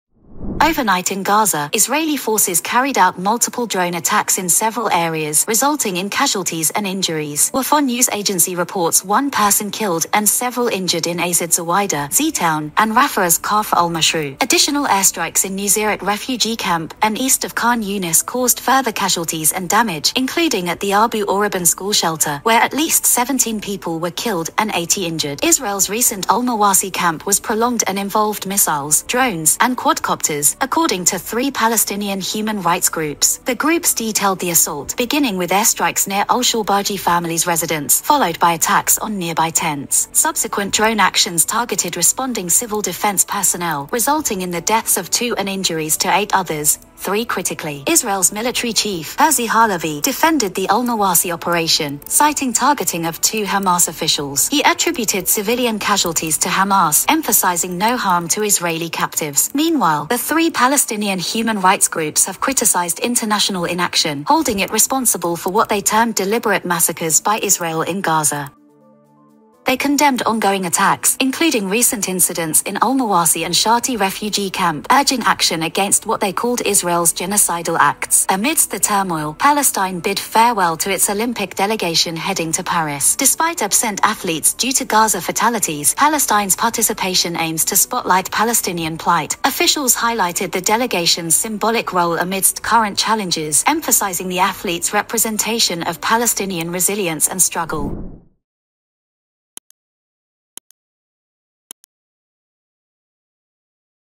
International Inaction Enables ‘Genocide’ in Gaza: Palestinian Rights Groups | Dawn News English
Three Palestinian human rights groups have blamed the international community for Israel’s “deliberate massacres” of civilians in Gaza, Al Jazeera reports.

“The international community’s failure to end the ongoing genocide” has enabled Israeli attacks, including the latest “horrific massacres” in the al-Mawasi area, which killed at least 90, and in the Shati refugee camp, where Israeli warplanes bombed Palestinians at prayer killing 22 people, the rights groups said.

“International inaction continues to embolden Israel to perpetrate international crimes,” the Palestinian Centre for Human Rights, Al-Haq, and the Al Mezan Center for Human Rights said in a joint statement.

“States arming and financing Israel’s genocidal acts are complicit in these crimes, including genocide,” they said.